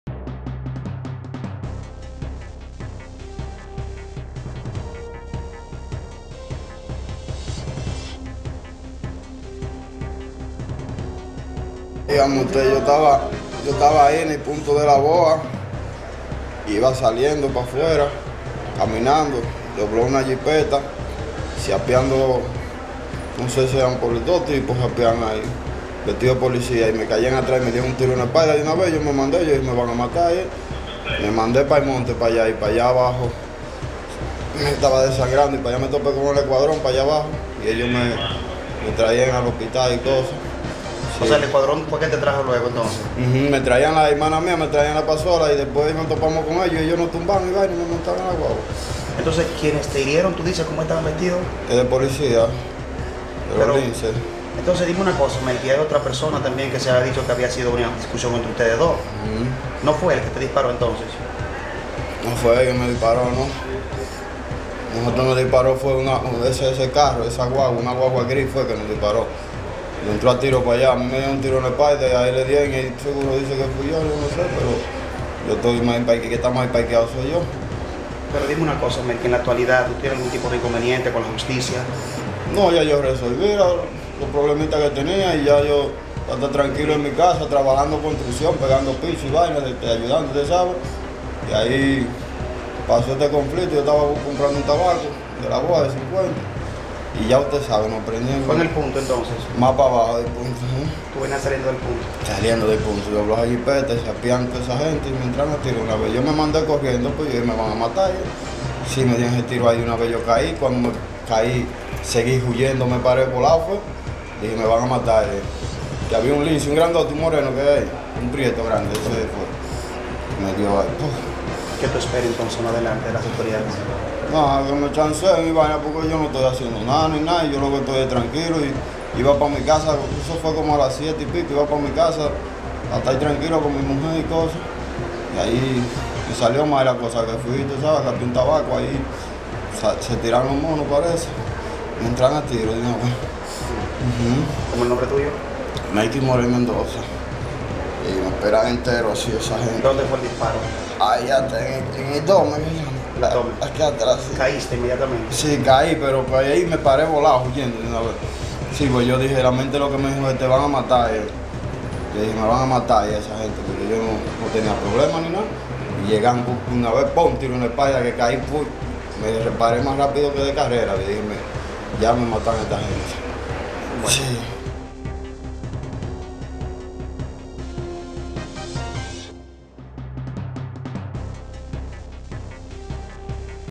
Hombre asegura agentes de la PN le dispararon durante incidente en Vista al Valle SFM
San Francisco de Macorís.- Uno de dos hombres que resultaron heridos de bala en el sector Vista al Valle, dijo que la situación no fue entre ellos, sino que fue la policía quién que le emprendió a tiros.

Postrado en una de las camas del Hospital Regional Universitario San Vicente de Paúl, Melkis Morel Mendoza, conocido como “Melky El Menor” aseguró que el disparo que recibió en la espalda se lo propinaron de forma sorpresiva agentes de la institución del orden.

Aseguró, que el hecho sucedió pasadas las siete de la noche del sábado 17 de junio, luego que el salió de comprar un “tabaco” de uno de los denominados punto de ventas de drogas ubicado en esa localidad.

Según Melkis, tras salir del punto los agentes abordo una camioneta de color negro lo persiguieron y le dispararon. Hasta el momento desconoce las razones del hecho, pues, aunque él tiene antecedentes penales, en la actualidad dice que se ha reivindicado.